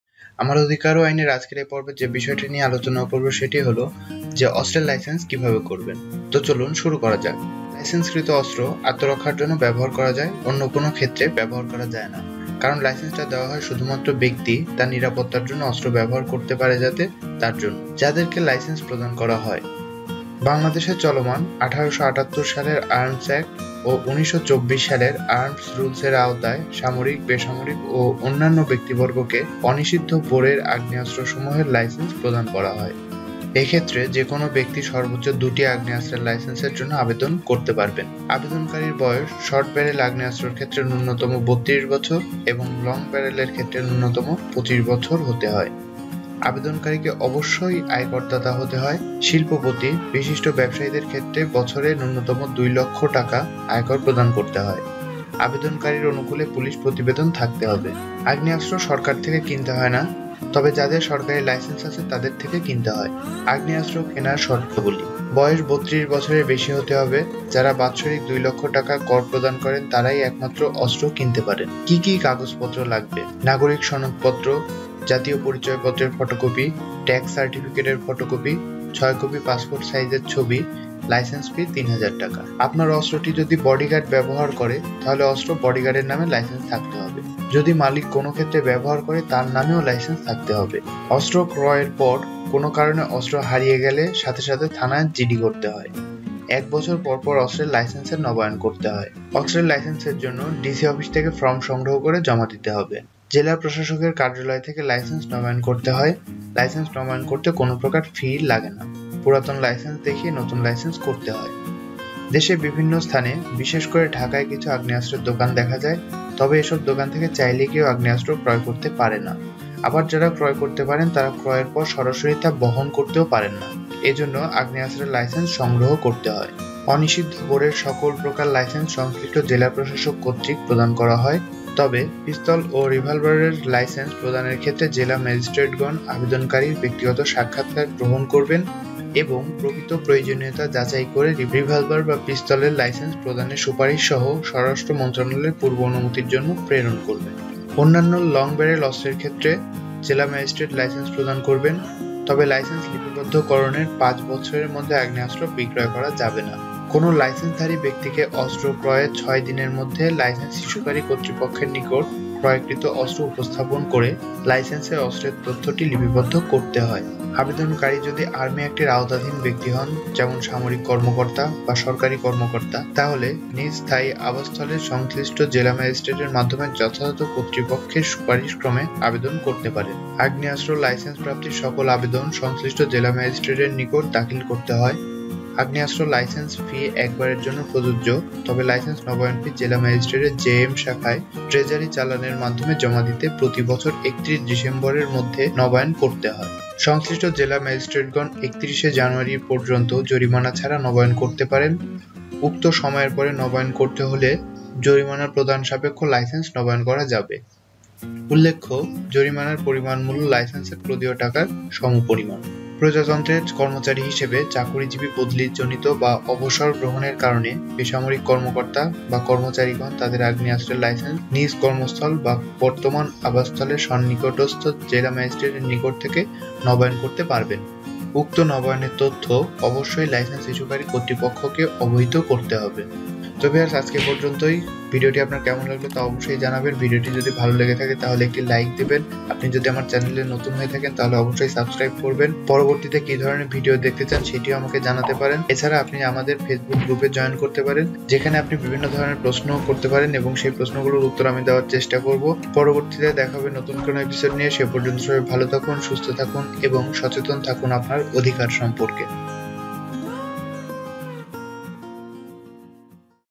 অস্ত্র কিনতে চাইলে জেনে নিন অস্ত্রের লাইসেন্স করার নিয়ম , অস্ত্র কোথায় পাওয়া যায় , অস্ত্রের দাম কত
লাইসেন্সকৃত অস্ত্র আত্মরক্ষার জন্য ব্যবহার করা যায়। অন্য কোন ক্ষেত্রে ব্যবহার করা যায় না। কারন লাইসেন্সটা দেওয়া হয় শুধুমাত্র ব্যক্তি তার নিরাপত্তার জন্য অস্ত্র ব্যবহার করতে পারে ।

বাংলাদেশে চলমান ১৮৭৮ সালের Arms Act ও ১৯২৪ সালের Arms Rules এর আওতায় সামরিক/বেসামরিক/অন্যান্য ব্যক্তিবর্গকে অনিষিদ্ধ বোরের আগ্নেয়াস্ত্রসমূহের লাইসেন্স প্রদান করা হয়। এক্ষেত্রে যেকোন ব্যক্তি সর্বোচ্চ দু’টি আগ্নেয়াস্ত্রের লাইসেন্সের জন্য আবেদন করতে পারবেন।

অস্ত্রের লাইসেন্স করার নিয়ম
অস্ত্র কোথায় পাওয়া যায়
অস্ত্রের দাম কত
অস্ত্র কিনতে চাই
বন্দুক এর দাম
বন্দুক কোথায় পাওয়া যায়

Video by Daniel absi from Pexels
Video by KML from Pexels